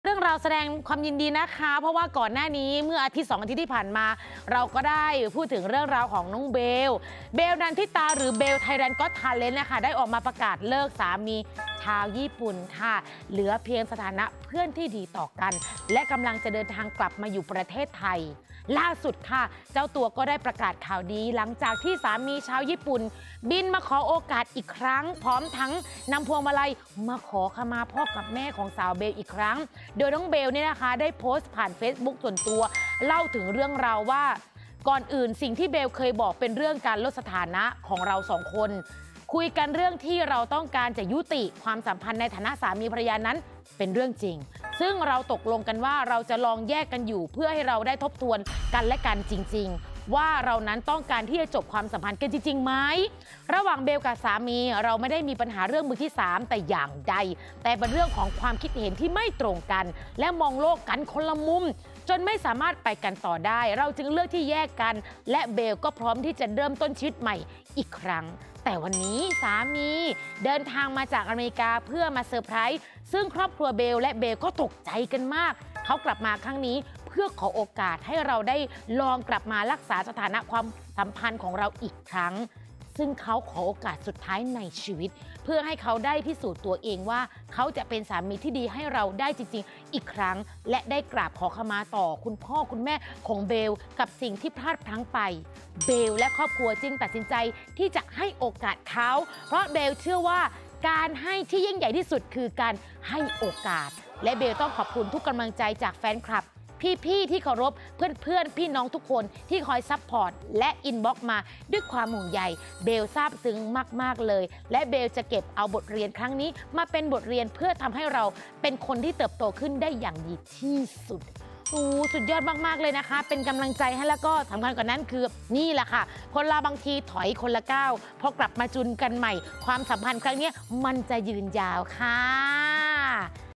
คืนดีแล้ว"เบลล์ นันทิตา" หลังสามีชาวญี่ปุ่นบินมาง้อ-ขอขมาพ่อแม่ I 14-09-66
คืนดีแล้ว"เบลล์ นันทิตา" หลังสามีชาวญี่ปุ่นบินมาง้อ-ขอขมาพ่อแม่